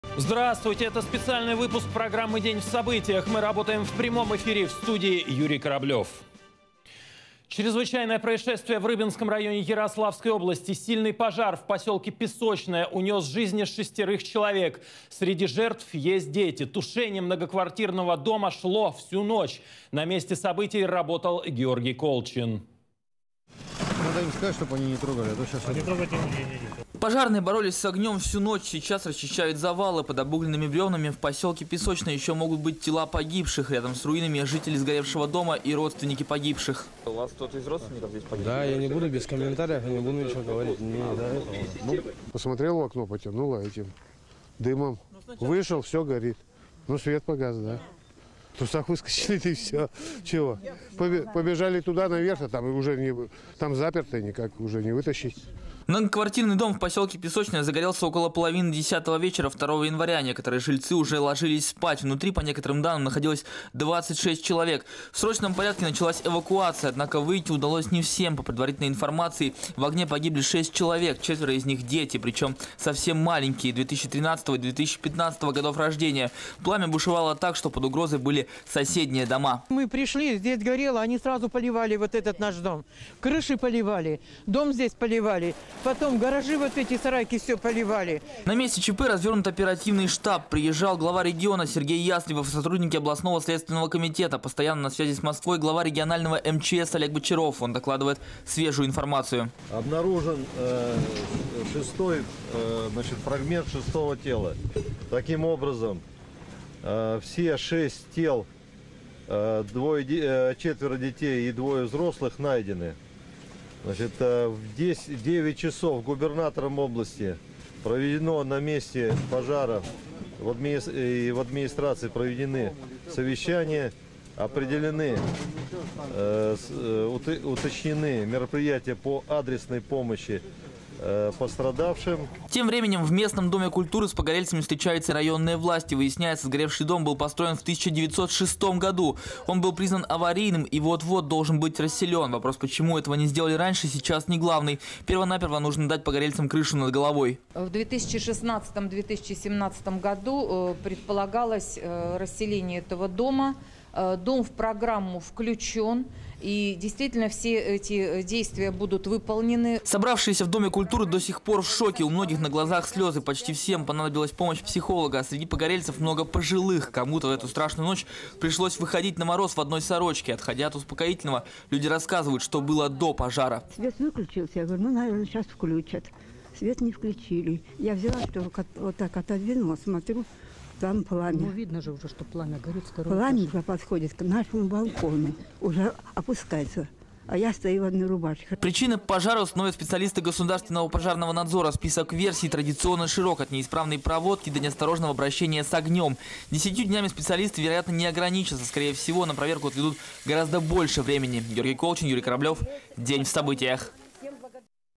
Пожар в Песочном: погибли четверо детей и двое взрослых
Чрезвычайное происшествие случилось в ночь на воскресенье, 3 января, в Ярославской области. Сильный пожар в поселке Песочное Рыбинского района унес жизни шестерых человек. Четверо погибших - дети, причем совсем маленькие - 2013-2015 годов рождения. Тушение многоквартирного дома шло всю ночь. С места событий Георгий Колчин.